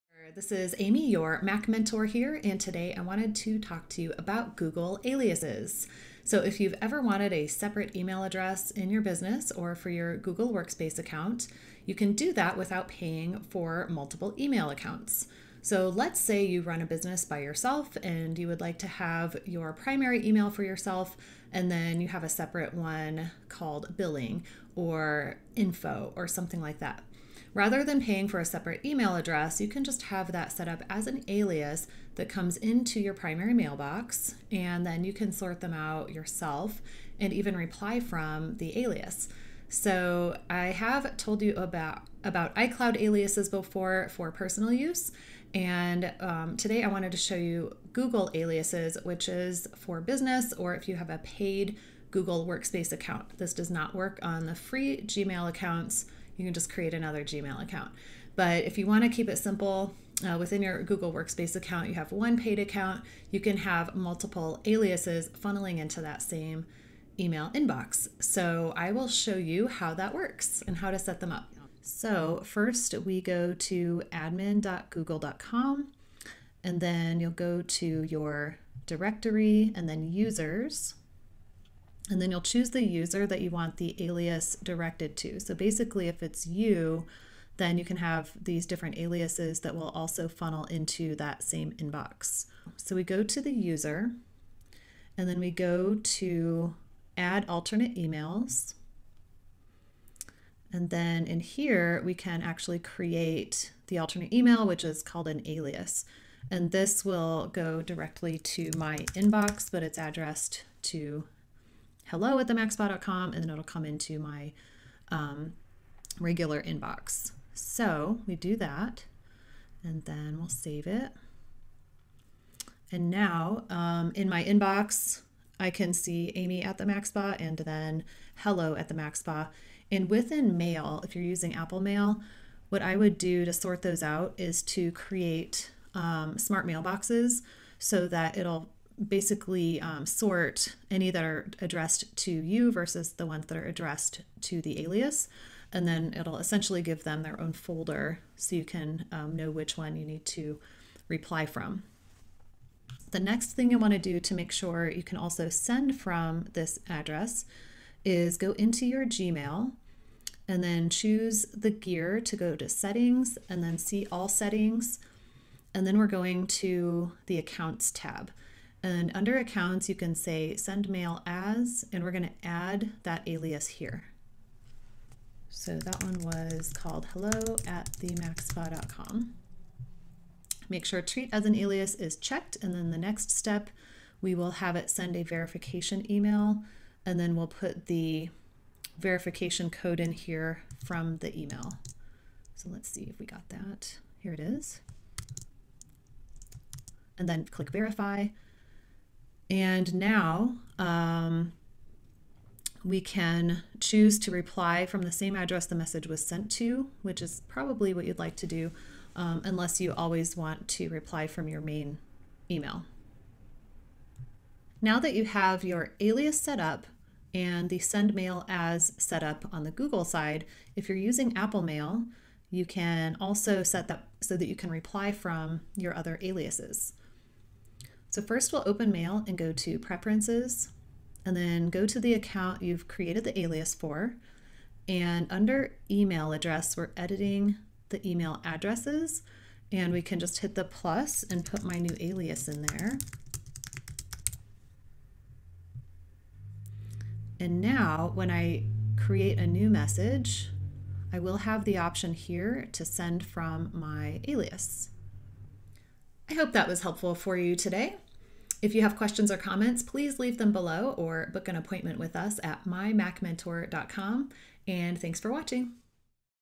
How to set up free Google email aliases
Did you know you can create email aliases for free from your Google Workspace account? Yes, you do need to pay for at least one email account to have this feature.

I'll show you how to set up your alias, then use it within the Gmail interface as well as Apple Mail.